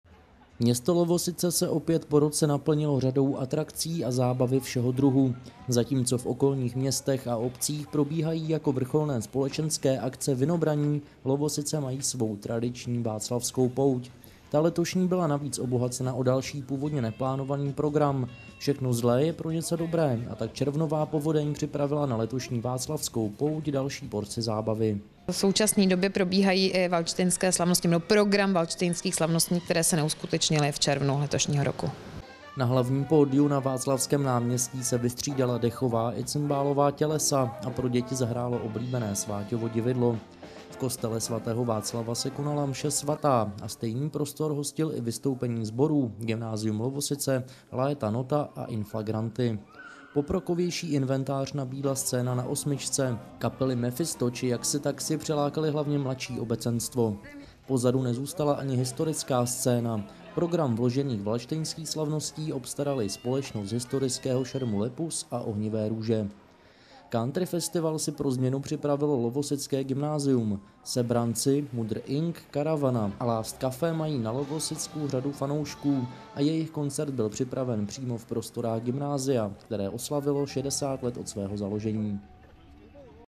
Svatováclavská pouť v Lovosicích
Město Lovosice se opět po roce naplnilo řadou atrakcí a zábavy všeho druhu. Zatímco v okolních městech a obcích probíhají jako vrcholné společenské akce vinobraní, Lovosice mají svou tradiční Václavskou pouť. Ta letošní byla navíc obohacena o další původně neplánový program. Všechno zlé je pro něco dobré a tak červnová povodeň připlavila na letošní Václavskou pouť další porci zábavy.